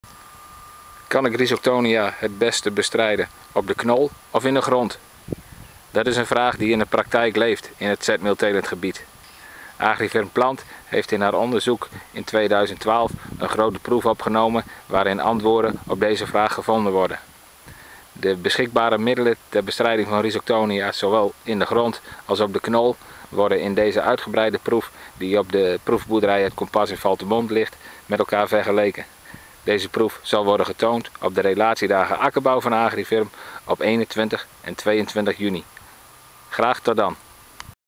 Bestrijding Rhizoctonia op de knol of in de grond?
Op de Akkerbouw relatiedagen van Agrifirm Plant in Valthermond op 21 en 22 juni wordt antwoord gegeven op de vraag of Rhizoctonia beter te bestrijden is op de knol of in de grond.